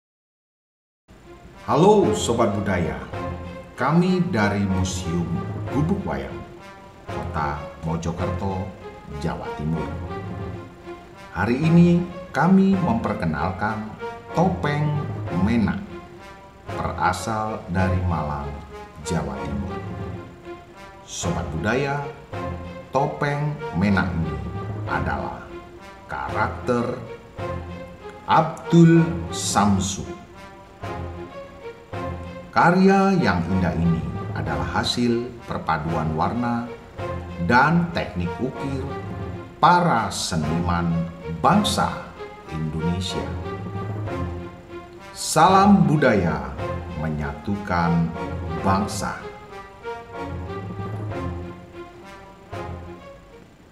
TOPENG MENAK EPS 2
Halo Sobat budaya! bertemu lagi dengan kita tim kreatif dari Gubug Inovatif. Kali ini kita akan memperkenalkan koleksi kita yaitu TOPENG MENAK MALANGAN koleksi dari Museum Gelar Indonesia Budaya (Ganesya) yang berlokasi di Malang.

Penasaran topengnya bagaimana? bisa datang ke Museum ya yang berlokasi di kompleks Hawai Waterpark Malang.

Salam Budaya menyatukan Bangsa 🙏🏻